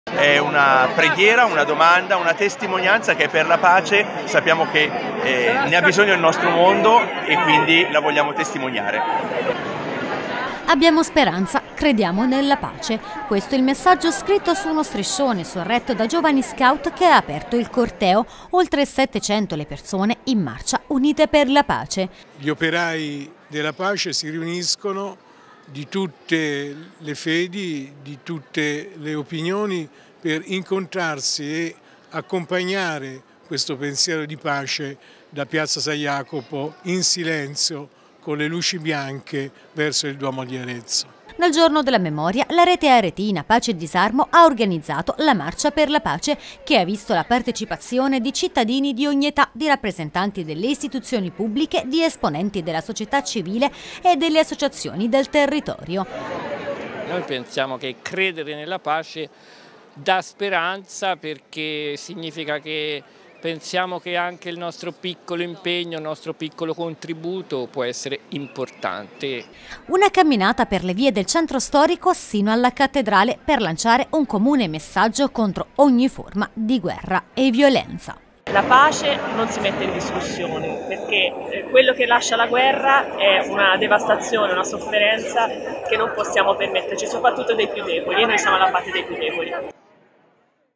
Marcia della Pace per le vie del centro di Arezzo
"Abbiamo speranza. Crediamo nella pace" è il messaggio scritto su uno striscione, sorretto da giovani Scout che hanno aprto il corteo, oltre 700 persone in marcia unite per la pace.
Nel giorno della Memoria la Rete Aretina Pace e Disarmo hanno organizzato la Pacia della Pace, che ha visto la partecipazione di cittadini di ogni età, di rappresentanti delle istituzioni pubbliche, di esponenti della società civile e dell’associazionismo del territorio.